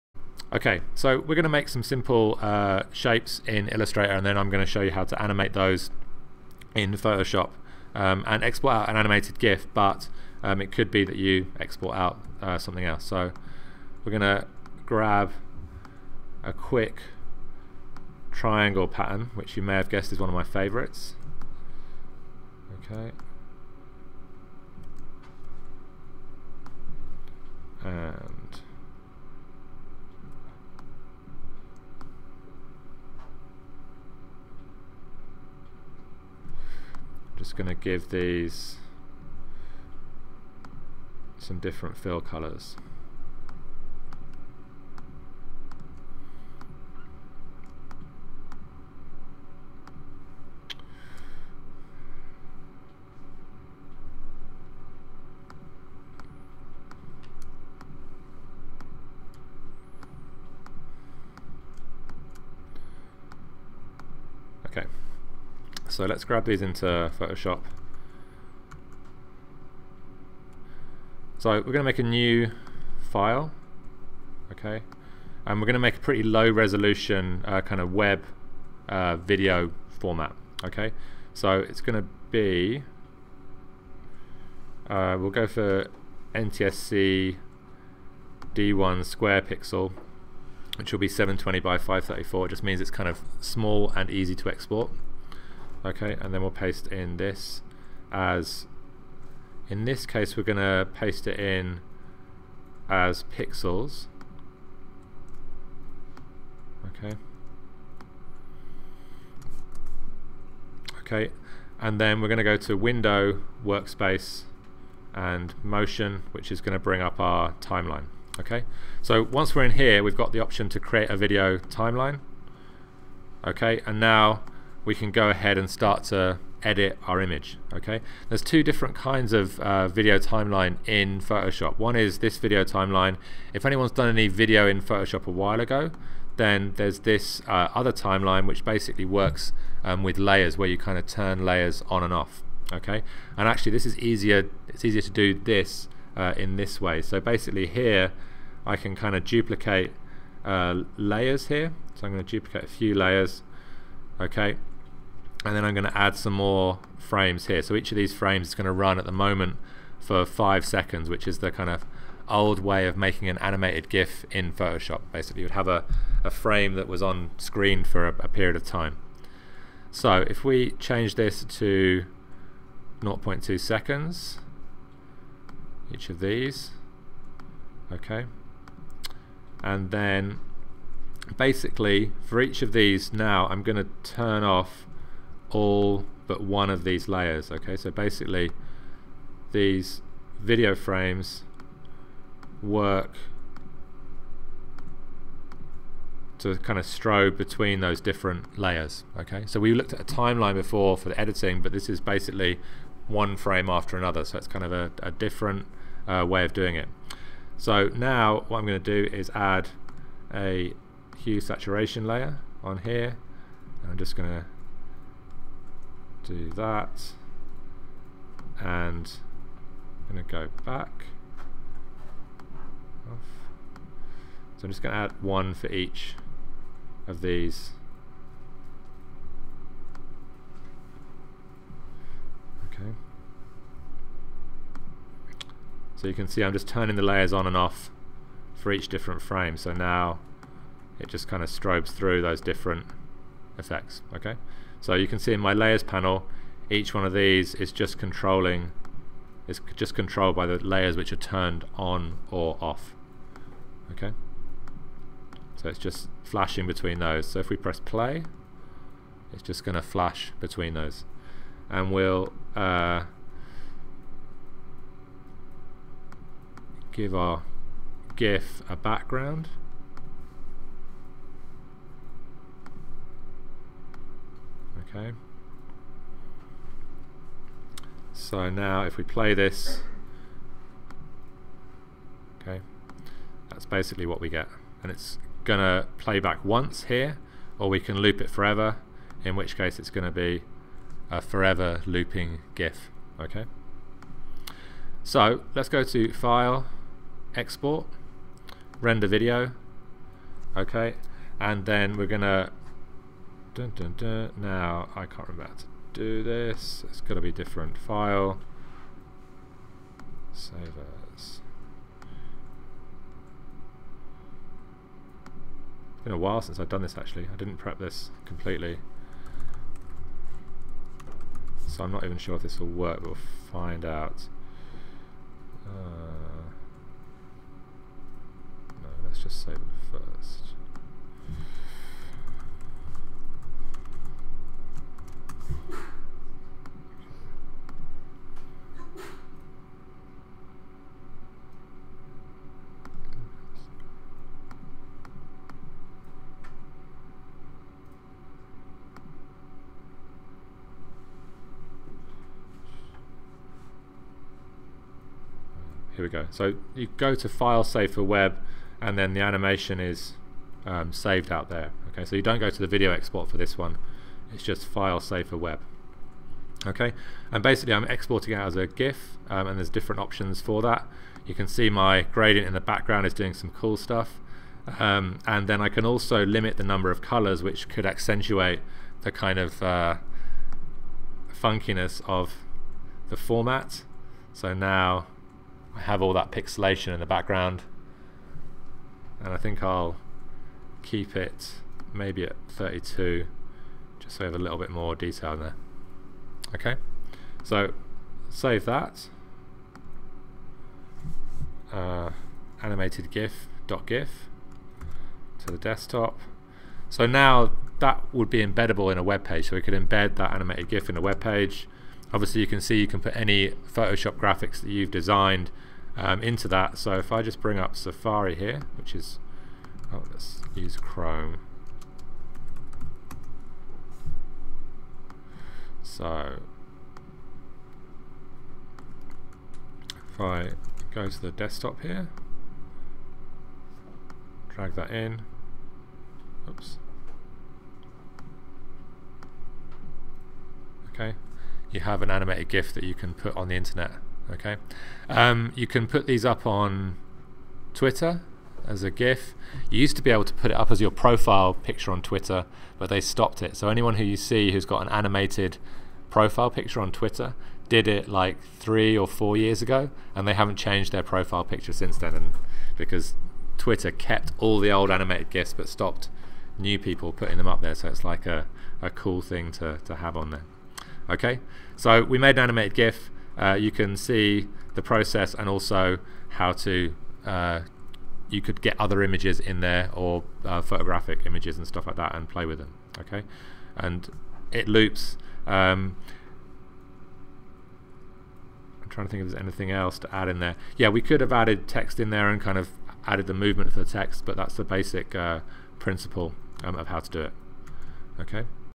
Photoshop: Create An Animated GIF (the old school way)
Using Adobe Illustrator and Photoshop learn how to make an animated GIF the old school way. You will run through how to create shapes, timings as well as exporting your GIF.